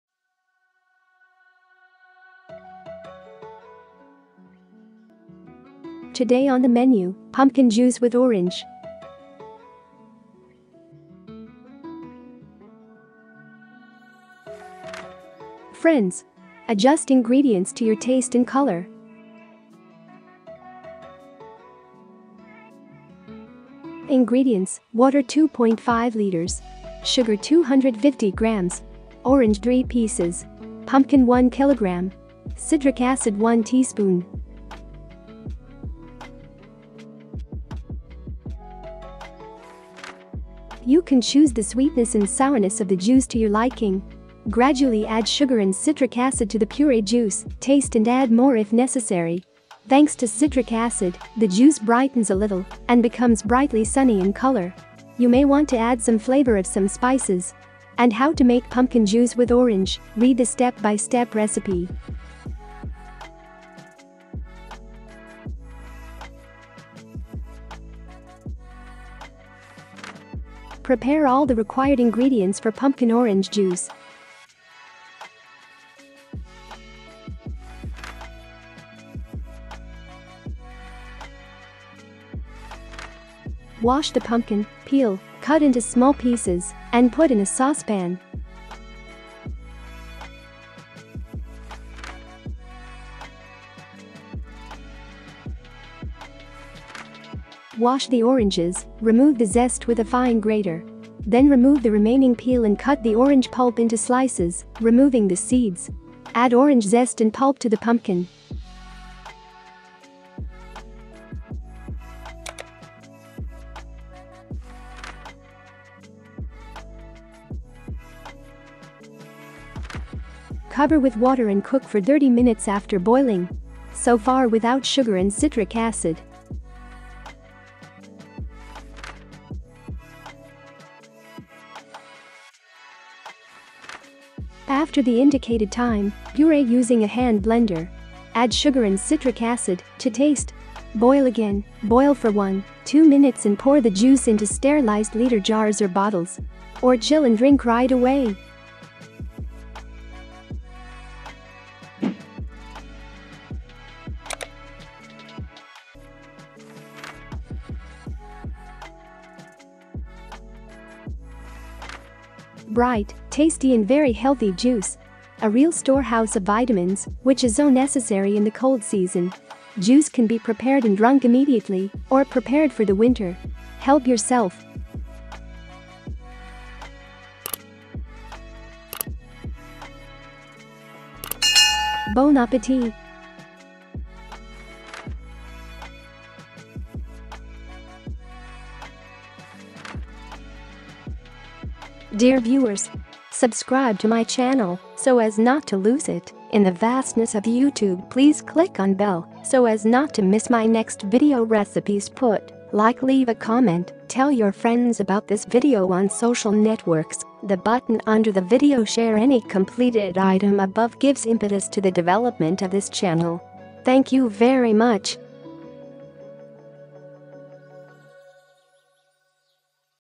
👉 Pumpkin Juice With Orange / 👉 Book of recipes / Bon Appetit
✅ Pumpkin Juice With Orange 👇

Ingredients:
* Water - 2.5 Liters
* Sugar - 250 Grams
* Orange - 3 Pieces
* Pumpkin - 1 Kilogram
* Citric acid - 1 teaspoon

You can choose the sweetness and sourness of the juice to your liking. Gradually add sugar and citric acid to the pureed juice, taste and add more if necessary. Thanks to citric acid, the juice brightens a little and becomes brightly sunny in color. You may want to add some flavor of some spices. And how to make pumpkin juice with orange, read the step-by-step recipe.

👉 Step by step cooking 👇

1. Prepare all the required ingredients for Pumpkin Orange Juice.

2. Wash the pumpkin, peel, cut into small pieces and put in a saucepan.

3. Wash the oranges, remove the zest with a fine grater. Then remove the remaining peel and cut the orange pulp into slices, removing the seeds. Add orange zest and pulp to the pumpkin.

4. Cover with water and cook for 30 minutes after boiling. So far without sugar and citric acid.

5. After the indicated time, purée using a hand blender. Add sugar and citric acid (to taste). Boil again, boil for one, 2 minutes and pour the juice into sterilized liter jars or bottles. Or chill and drink right away!

6. Bright, tasty and very healthy juice! A real storehouse of vitamins, which is so necessary in the cold season. Juice can be prepared and drunk immediately, or prepared for the winter. Help yourself!

👉 GOOD LUCK TO ALL AND DO NOT GIVE UP IN YOUR CULINARY BEGINNINGS !!!